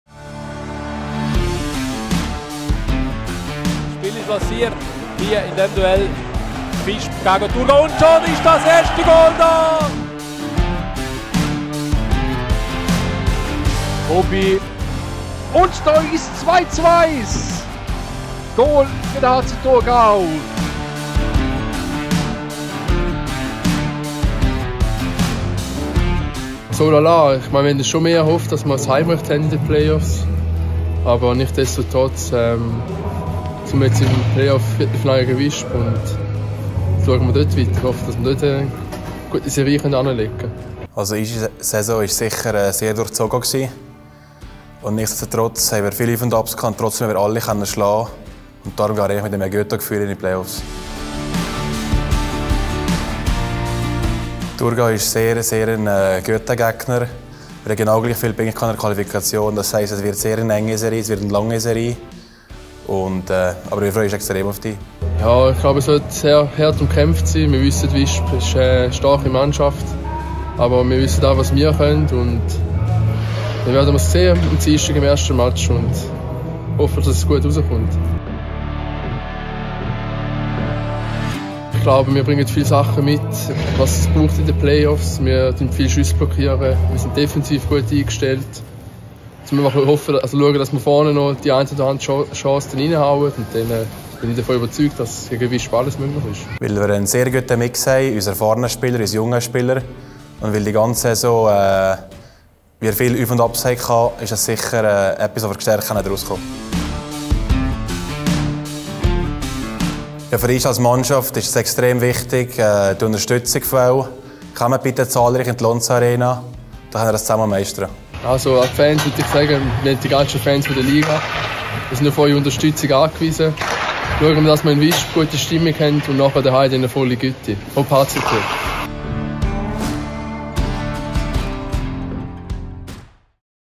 EHC Visp vs. HC Thurgau | Swiss League Playoff Preview
Mit dem EHC Visp und dem HC Thurgau treffen im Viertelfinal der Swiss League zwei Teams aufeinander, welche in der Regular Season exakt gleich viele Punkte erkämpft haben. Das verspricht also viel Spannung für die Serie!  Während der Regular Season konnte der HC Thurgau drei der fünf Spiele gegen Visp gewinnen. Können die Walliser das Blatt nun wenden?
Luis Janett vom HC Thurgau und Raphael Kuonen vom EHC Visp liefern euch Argumente für ihre Teams.